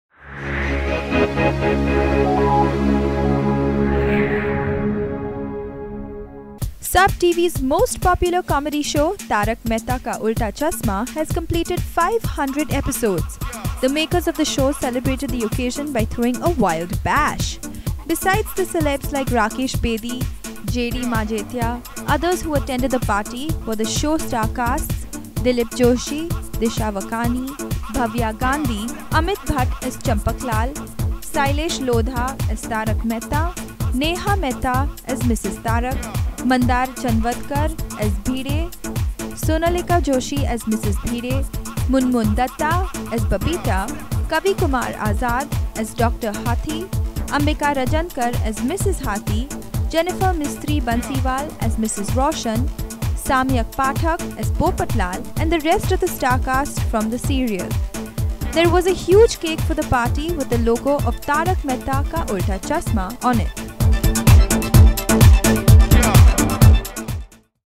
Tarak Mehta Ka Ootla Chashma Wild Party Celebrations
A huge wild party was thrown for the cast and crew of Tarak Mehta Ka Oolta Chashma. They celebrated the completion of 500 episodes.